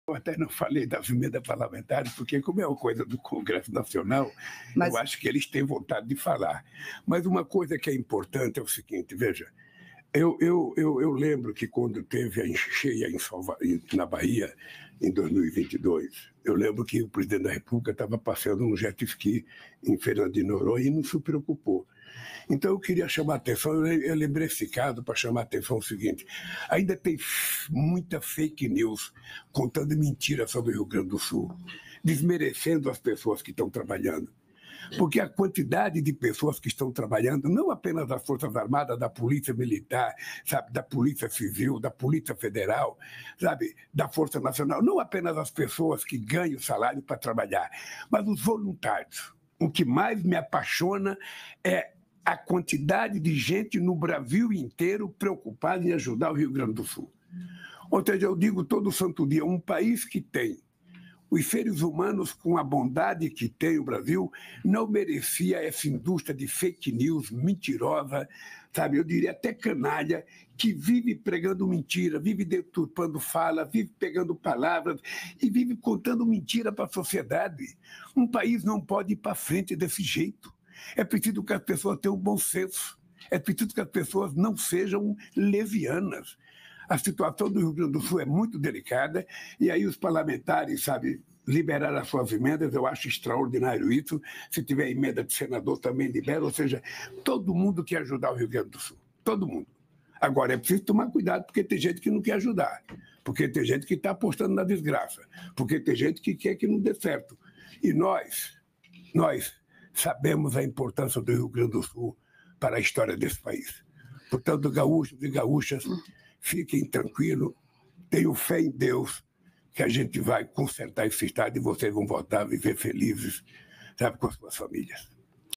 Lula diz que fake news sobre Rio Grande do Sul desmerecem quem está ajudando: 'Aposta na desgraça'
O presidente Luiz Inácio Lula da Silva discute nesta terça-feira (7) a situação de calamidade no Rio Grande do Sul, os planos do governo e outros assuntos